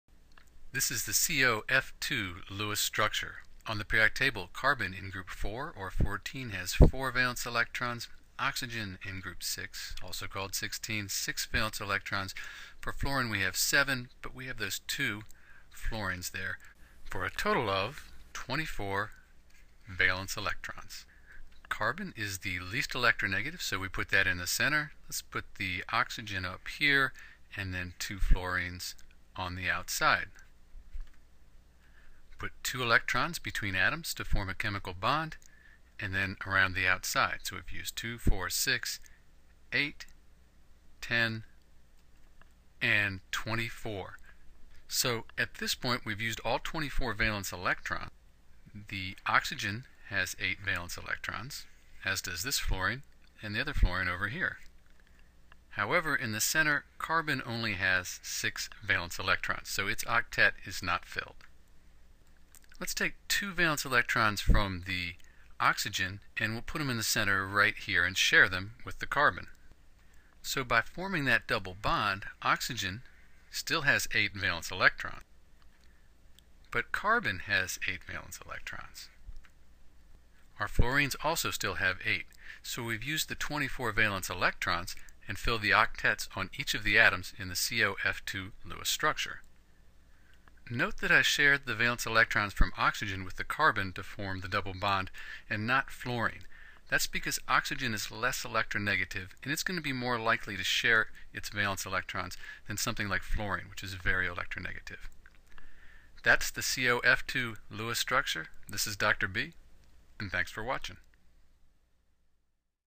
COF2 Lewis Structure - How to Draw the Lewis Structure for COF2
A step-by-step explanation of how to draw the COF2 Lewis Dot Structure (Carbonyl fluoride).

For the COF2 structure use the periodic table to find the total number of valence electrons for the COF2 molecule. Once we know how many valence electrons there are in COF2 we can distribute them around the central atom with the goal of filling the outer shells of each atom.

In the Lewis structure of COF2 structure there are a total of 24 valence electrons. COF2 is also called Carbonyl fluoride.

Steps to Write Lewis Structure for compounds like COF2
1. Find the total valence electrons for the COF2 molecule.
2. Put the least electronegative atom in the center. Note: Hydrogen (H) always goes outside.
3. Put two electrons between atoms to form a chemical bond.
4. Complete octets on outside atoms.
5. If central atom does not have an octet, move electrons from outer atoms to form double or triple bonds.

Lewis Structures, also called Electron Dot Structures, are important to learn because they help us understand how atoms and electrons are arranged in a molecule, such as Carbonyl fluoride. This can help us determine the molecular geometry, how the molecule might react with other molecules, and some of the physical properties of the molecule (like boiling point and surface tension).